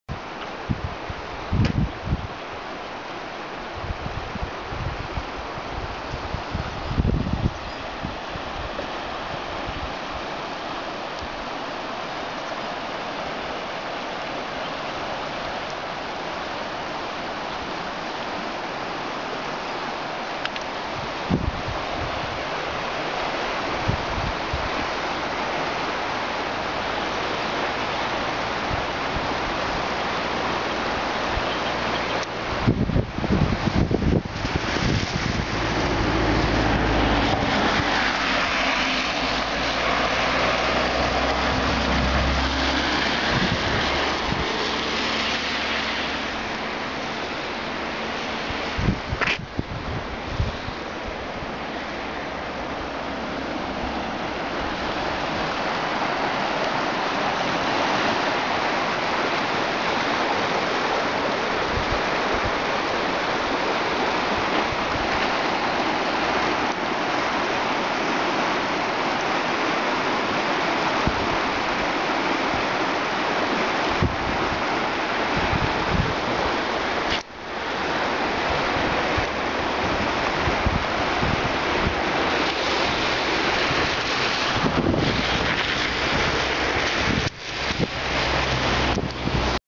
وادي دادس بعد تساقط الأمطار يوم 4 مارس 2013
شريط يظهر أمتلاء حقينة وادي دادس بعد التساقطات المطرية التي عرفتها المنطقة ليلة وصبيحة يوم 4 مارس 2013